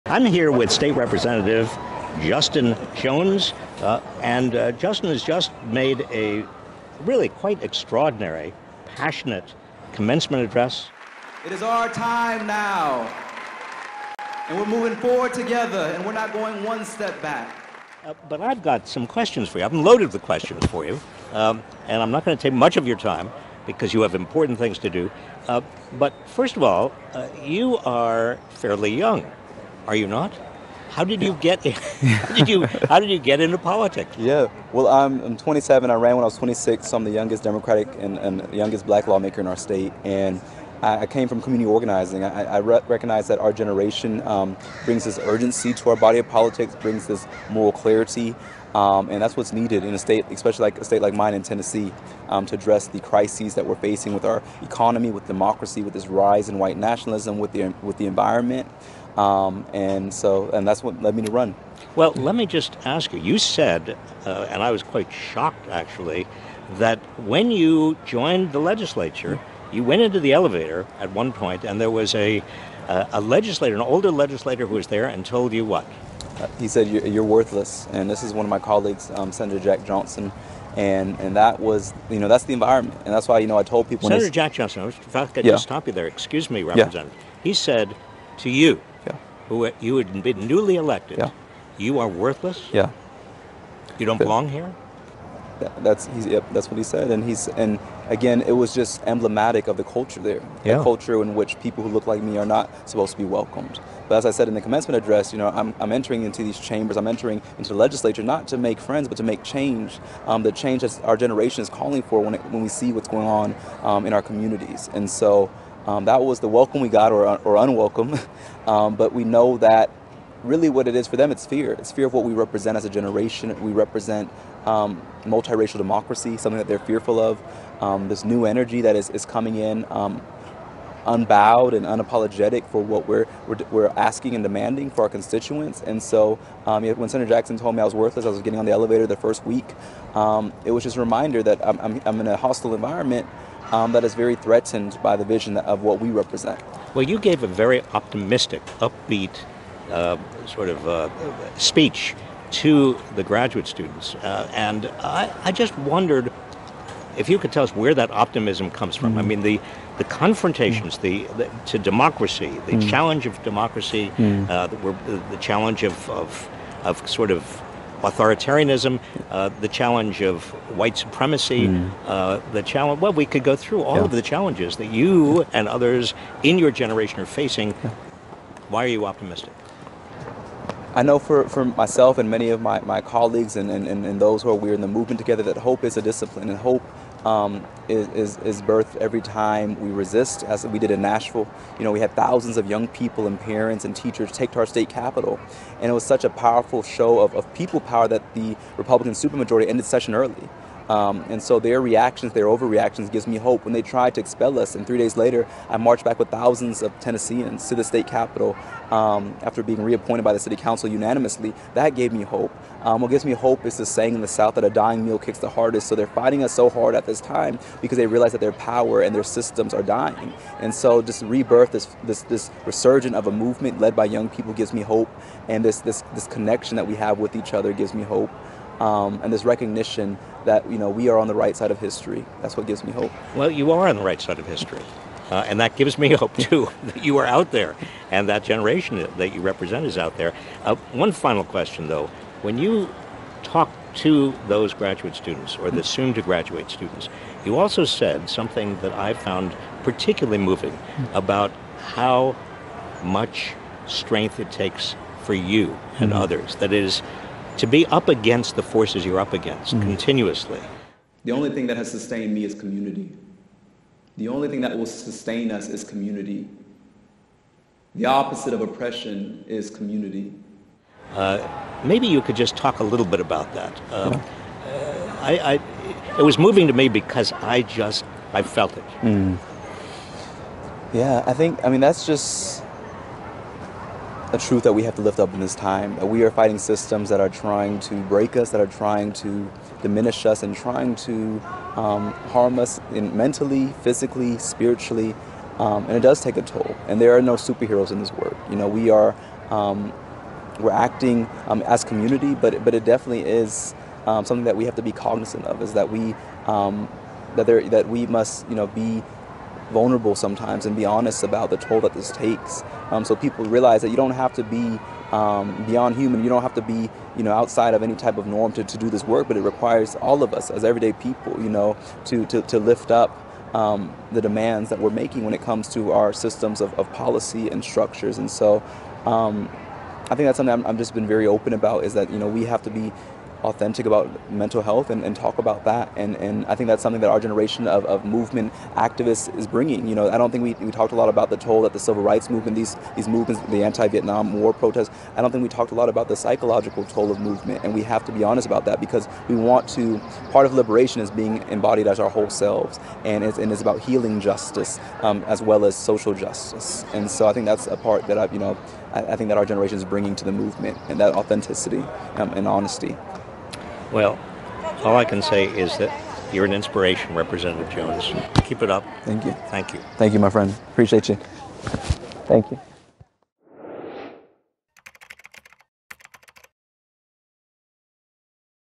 In Conversation with TN State Representative Justin Jones
My full conversation with Tennessee State Representative Justin Jones, who was recently reinstated after being expelled from the Tennessee House of Representatives for standing up for democracy.